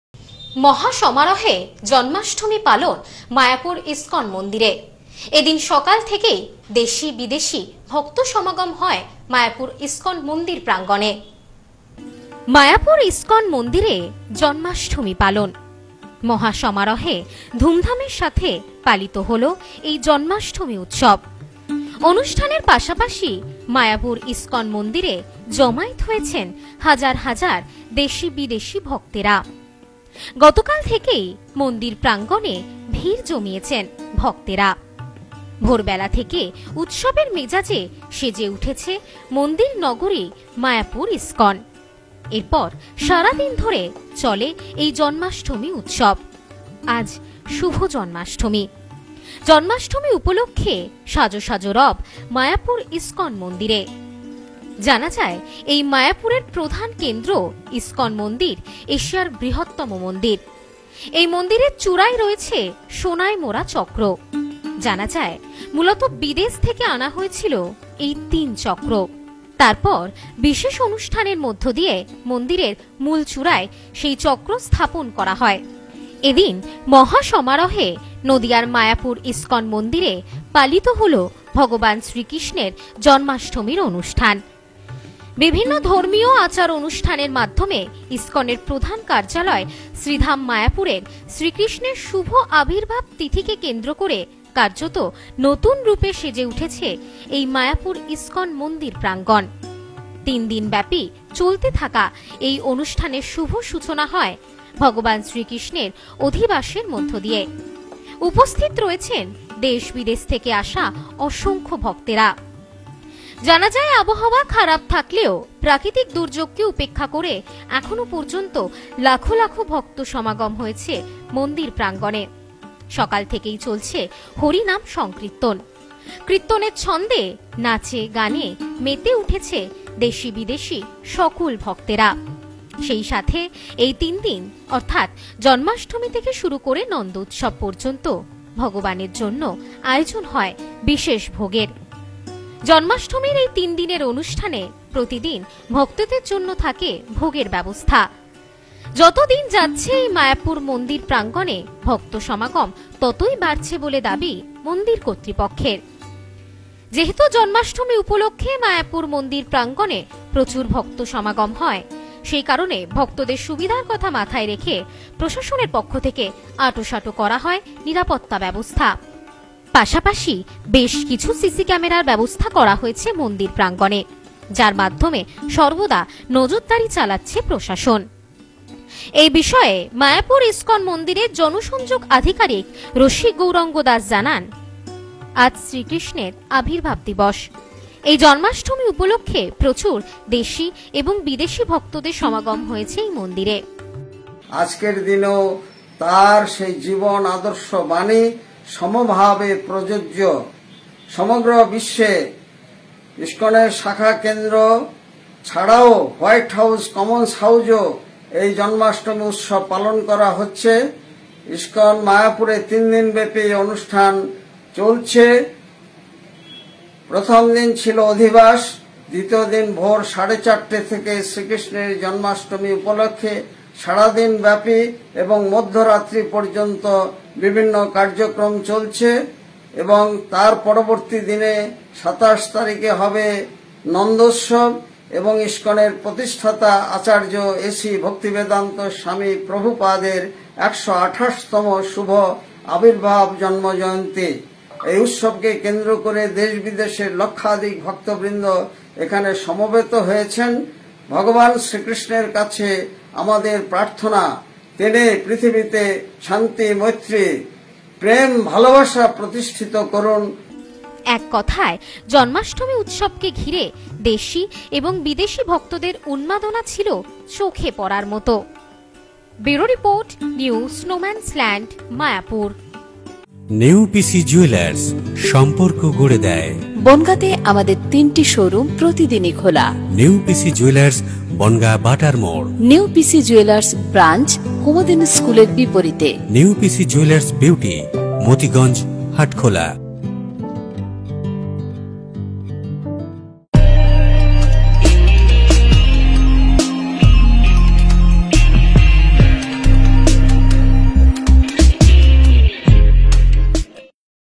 মহাসমারহে জন্মাষ্টমী পালন মায়াপুর ইসকনে।
মহাসমারহে জন্মাষ্টমী পালন মায়াপুর ইসকনে। মন্দিরে ভিড় জমিয়েছেন হাজার হাজার দেশি-বিদেশি ভক্ত।